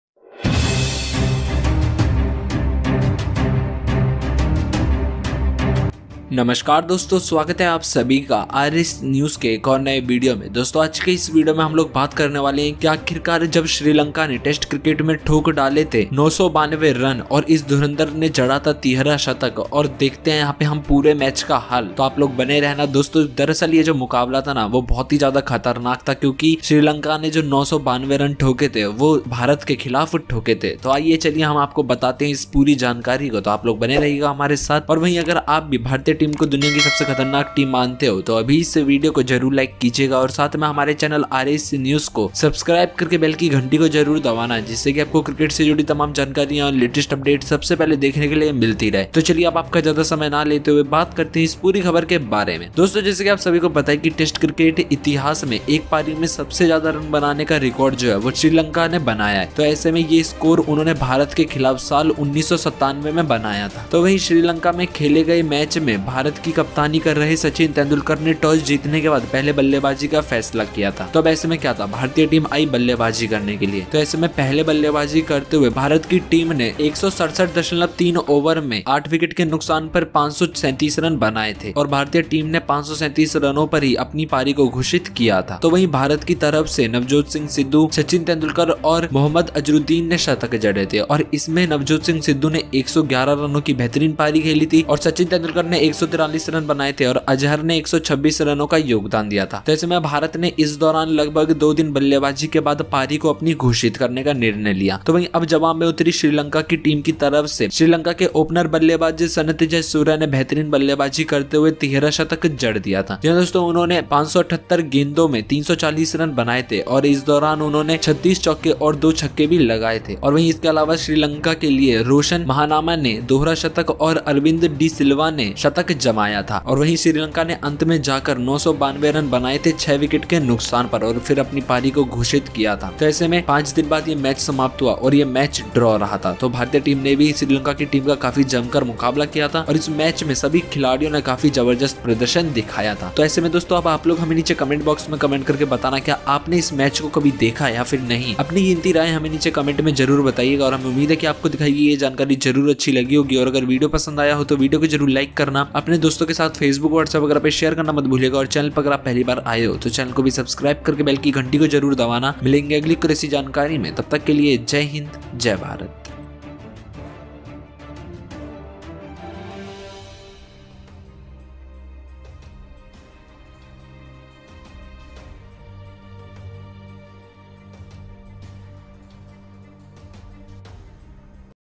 देखिये,जब श्रीलंका ने भारत के खिलाफ टेस्ट में ठोके थे 952 रन,इस धुरंधर ने जड़ा था तिहरा शतक,देखेमैच
ABOUT CRICKET NEWS:

Sri Lanka holds the record for the highest runs scored in an innings in Test cricket history. He scored this score against India in 1997. Sachin Tendulkar, who captained India in the match played in Sri Lanka, decided to bat first after winning the toss.

Batting of india
Batting first, the India team declared the innings after scoring 167.3 overs 537/8.
From India, Navjot Sidhu, Sachin Tendulkar and Mohammad Azharuddin had scored centuries. In this, Sidhu contributed 111 runs, Sachin 143 and Azhar contributed 126 runs.

Jayasuriya had scored a triple century
Sri Lanka opener batsman Sanath Jayasuriya hit a triple century while batting brilliantly in front of India's first innings score.
He scored 340 runs in 578 balls, and during this he hit 36 ​​fours and 2 sixes.
Apart from this, Roshan Mahanama had a double century and Arvind de Silva had a century for Sri Lanka. Sri Lanka finally declared the innings at the score of 952/6. By then even 5 days of the match had ended, and with that the match ended on a draw.